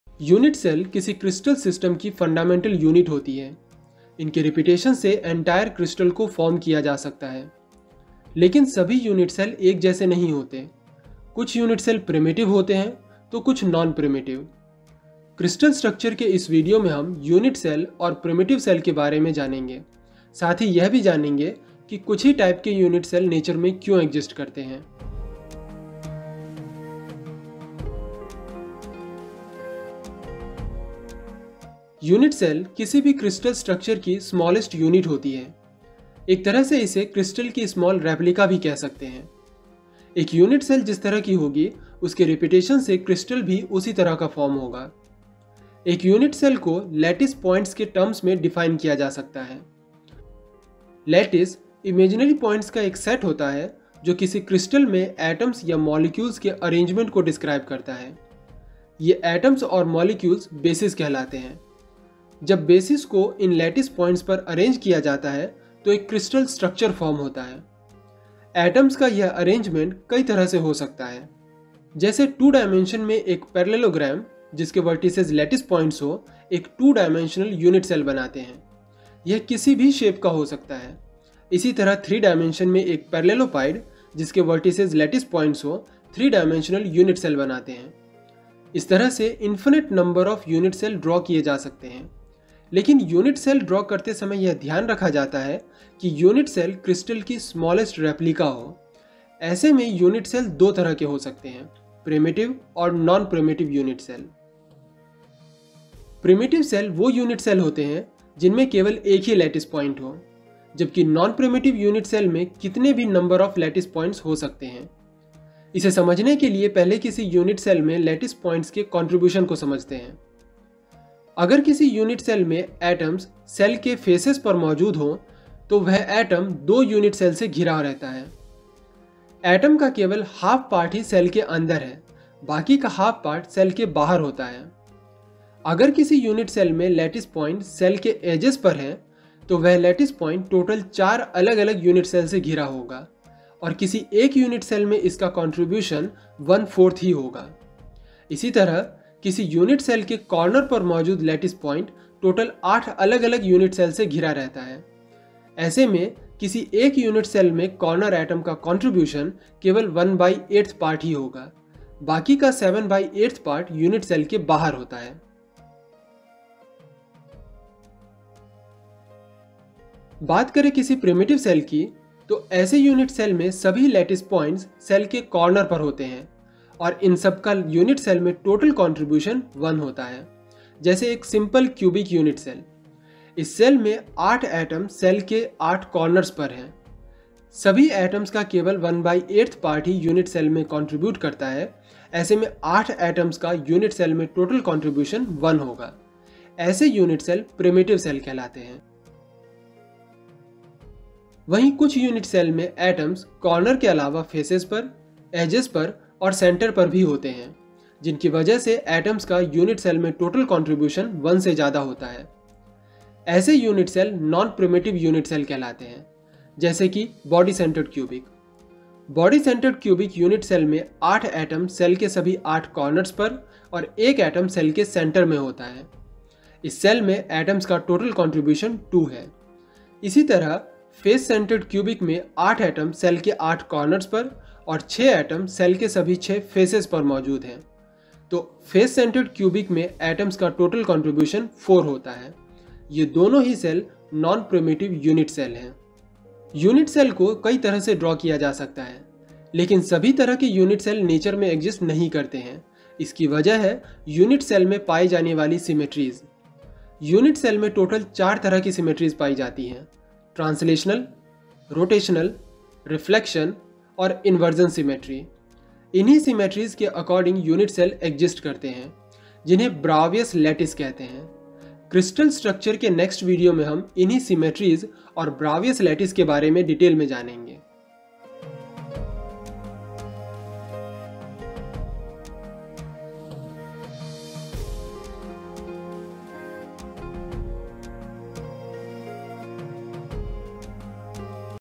Unit Cell | Crystal Structure | In Hindi
The unit cell is the fundamental unit of a crystal. But not all the unit cells are the same. Some of them are primitive cell, while some are non-primitive cell.